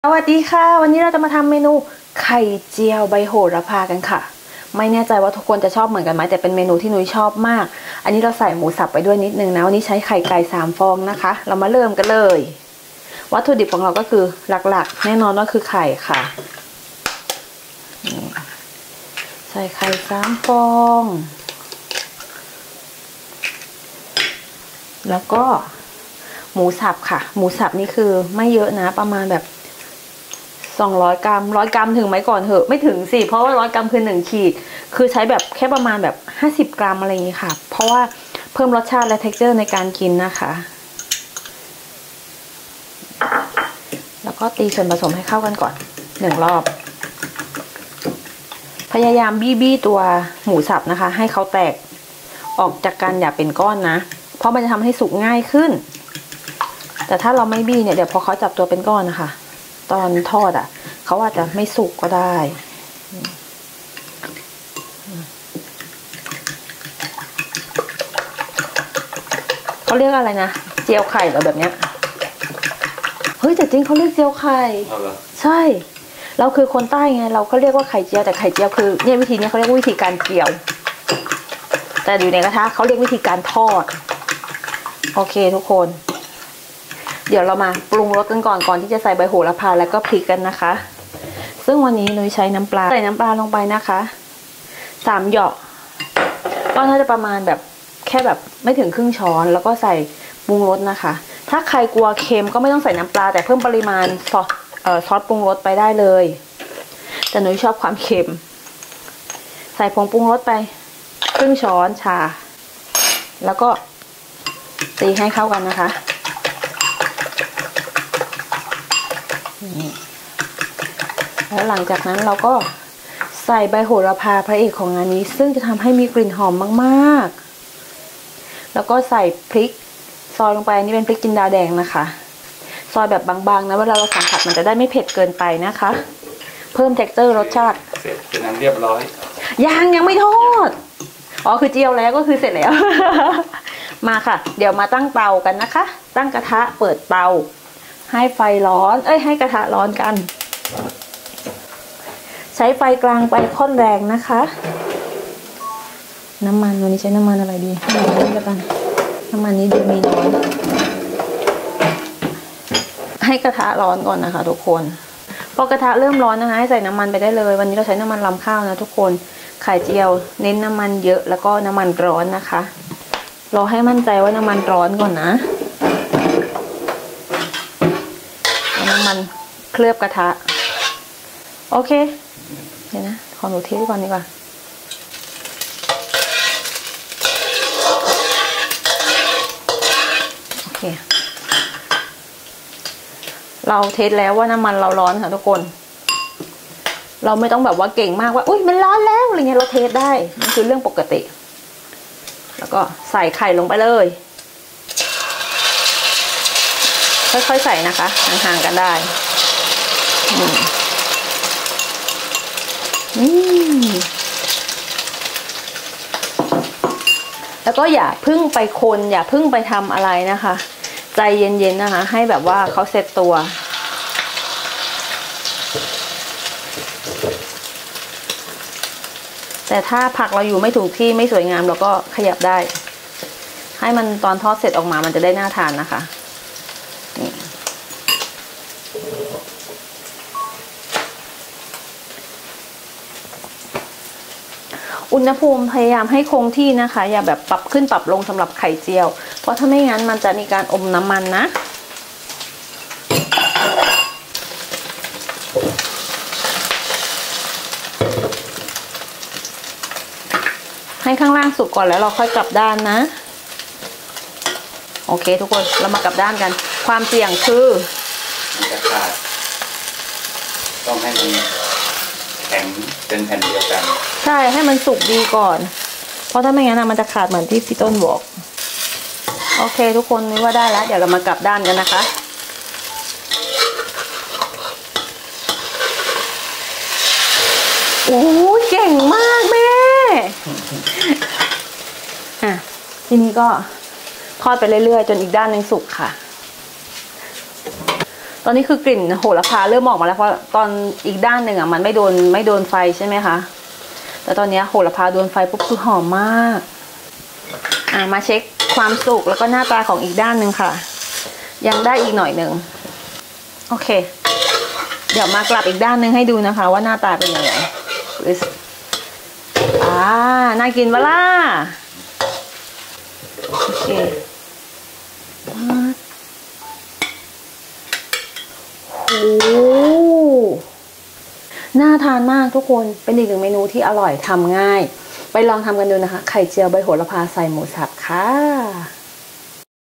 ไข่เจียวใบโหระพา/ Basil omelette ทำง่ายอร่อยมาก ไข่กรอบนอกนุ่มในไม่อมน้ำมัน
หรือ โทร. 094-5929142